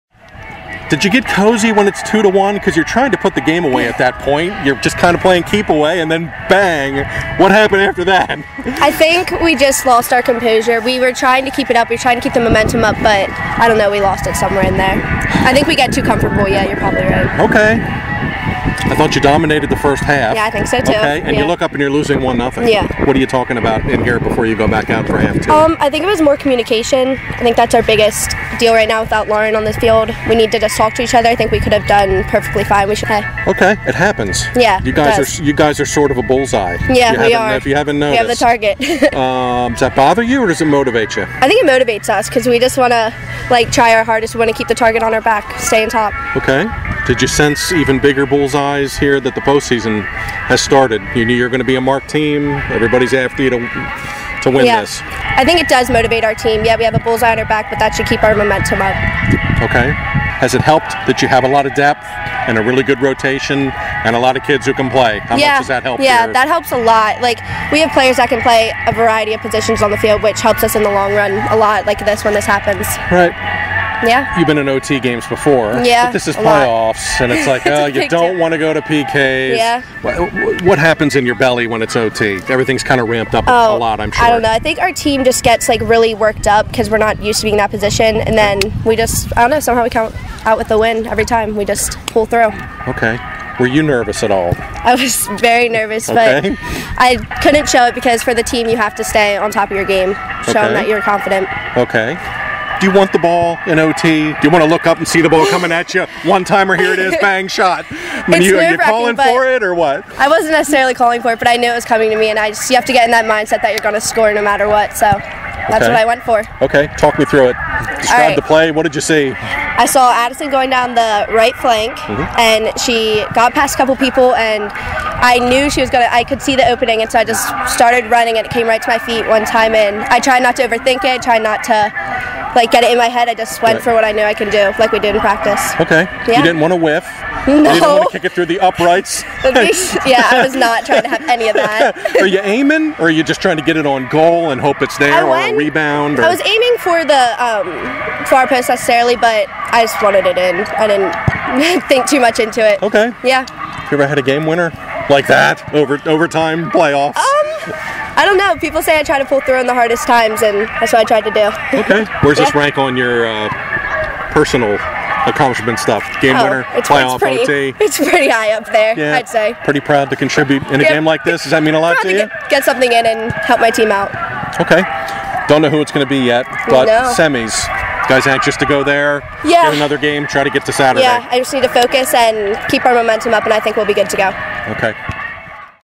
Ashlee Hoover Podcast interview
Interview with Conestoga Valley's Ashlee Hoover, after she scored the game-winning goal in the Buckskins' 4-3 OT win over Donegal in the league quarterfinals 5-8-12 at Warwick.